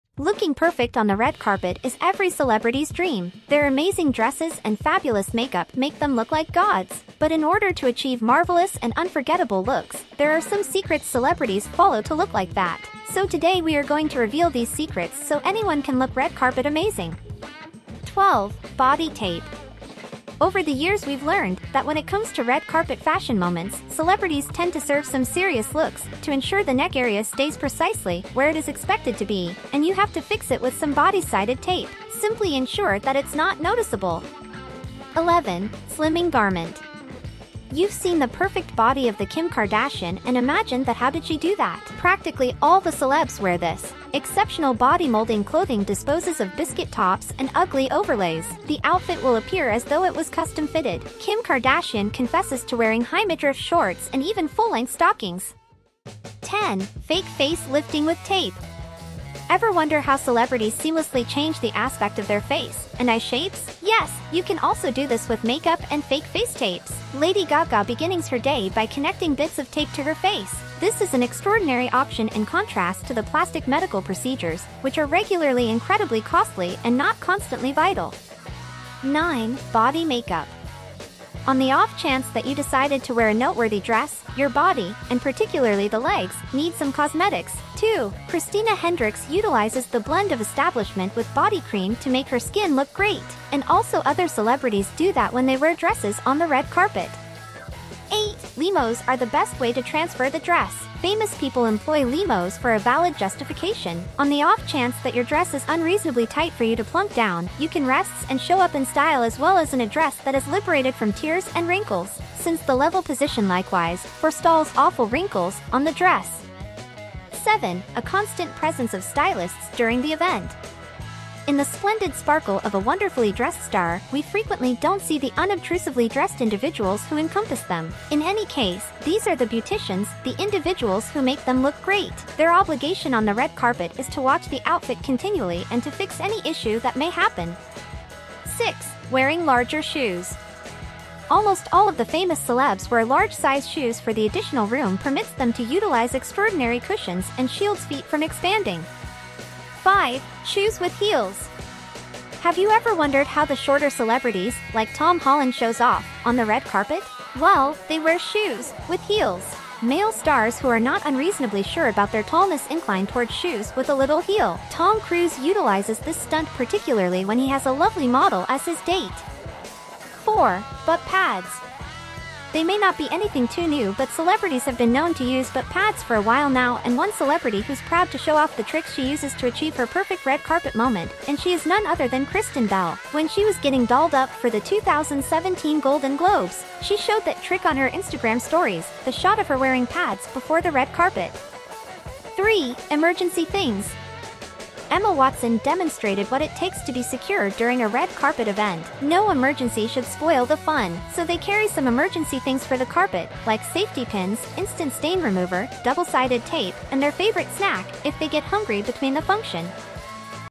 Inside the MET Gala: Secrets Exposed!
WATCH: Inside the MET Gala: Secrets Exposed!
Looking perfect on the red carpet is every celebrity’s dream. Their amazing dresses and fabulous makeup make them look like Gods. But in order to achieve marvelous and unforgettable looks, there are some secrets celebrities follow to look like that. So today we are going to reveal these secrets so anyone can look red carpet amazing.